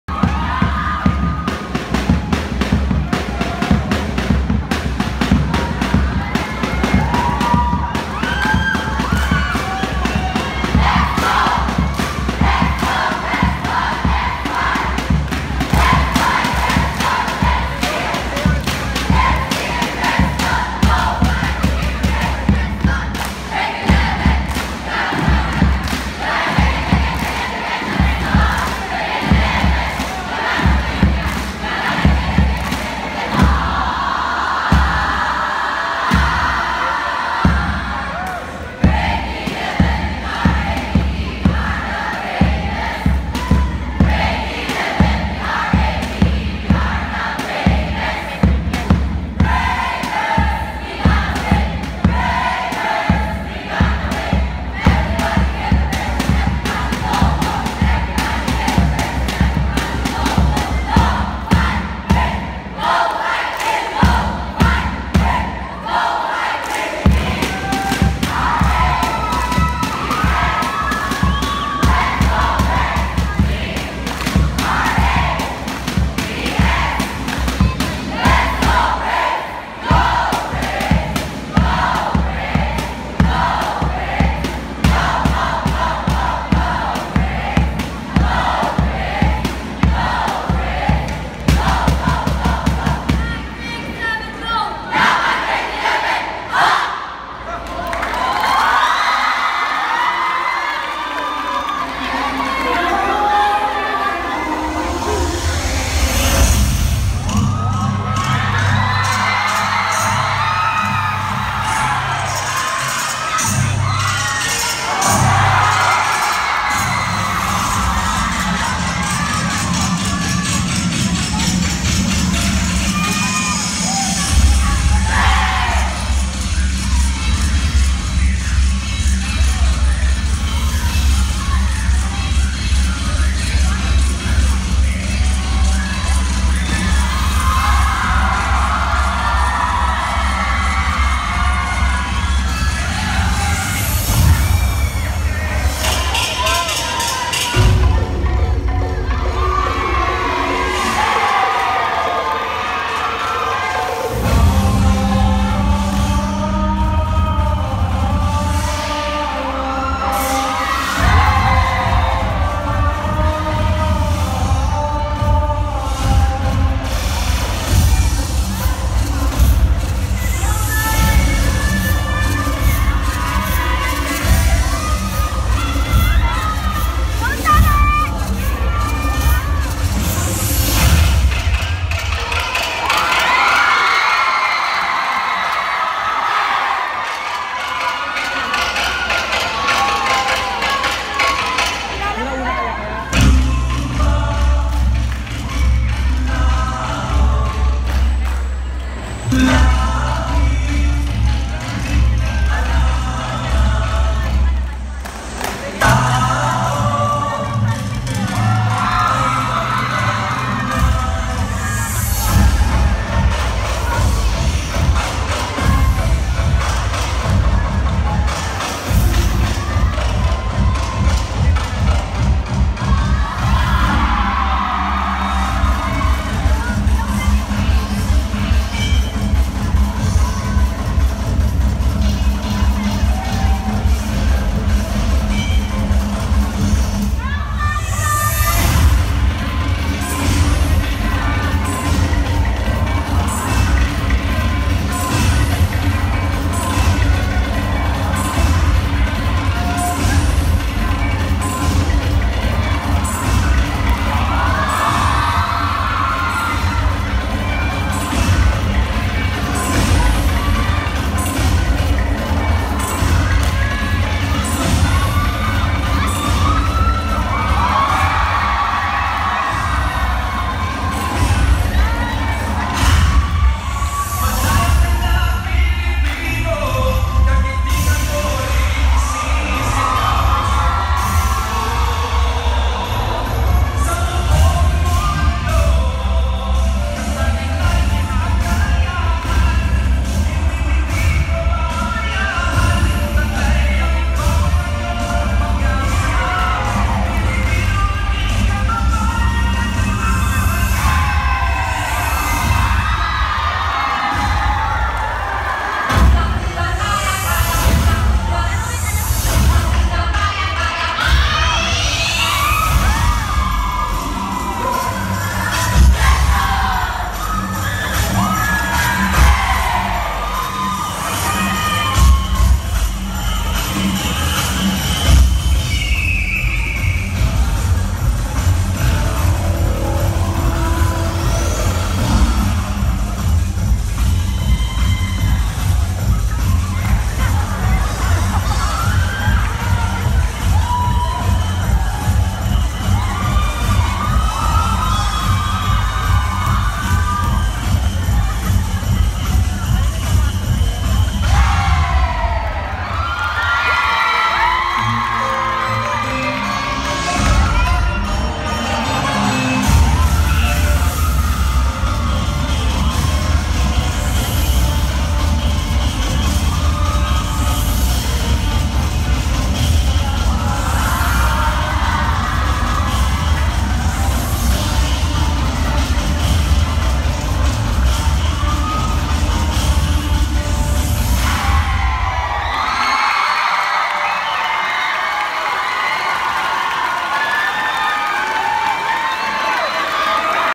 Treston Braves Cheerdance Champion A.Y. 2017-2018
Watch the performance of Grade 11 Braves in the Sports Festival @ Treston International College. (ctto)